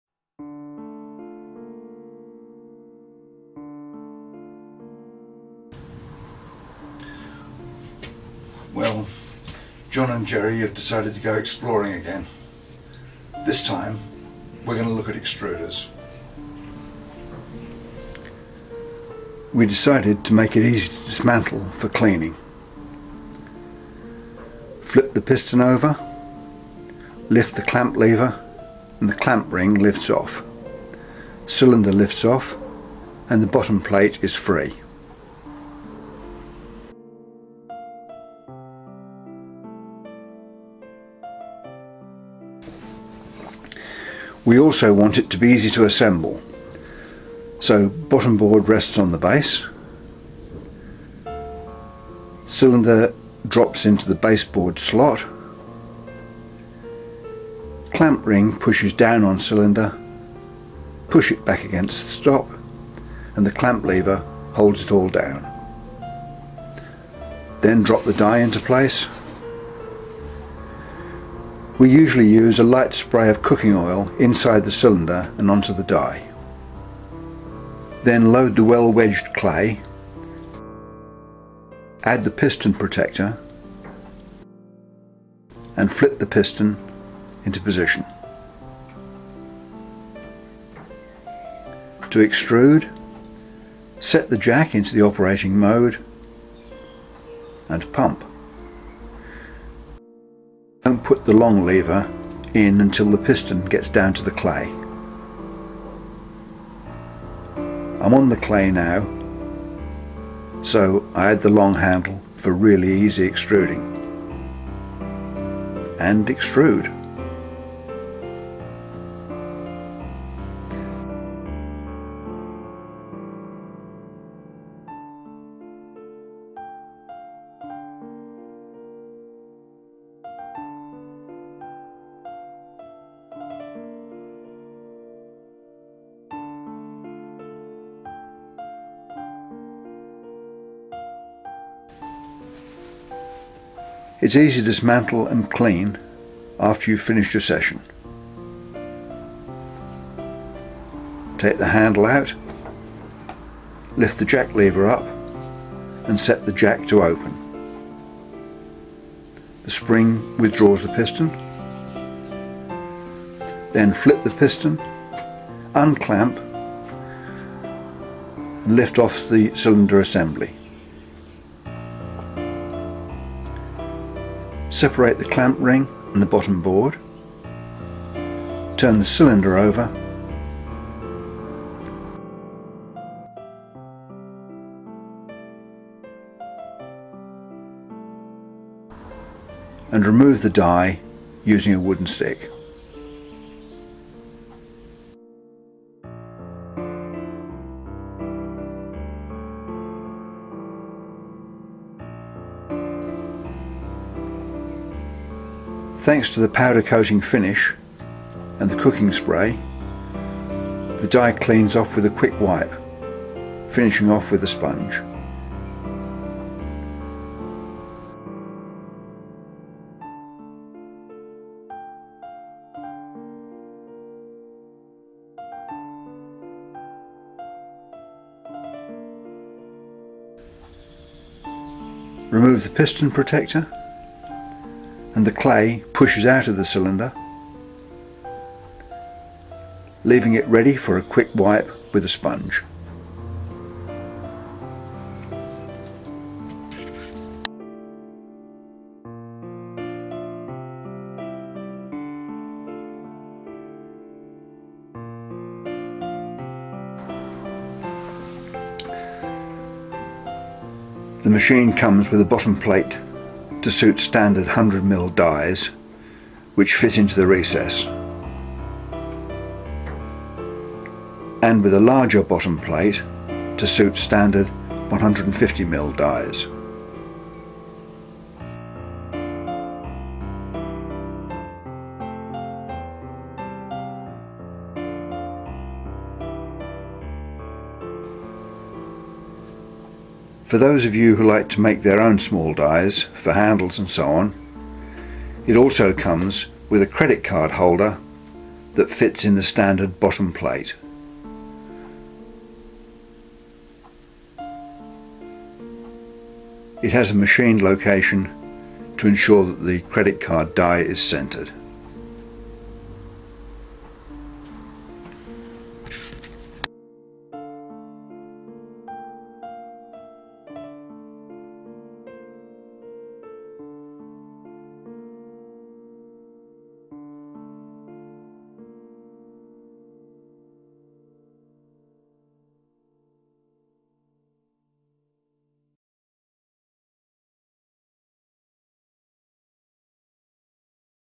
Jerry and John's new extruder
A brand new design of clay extruder.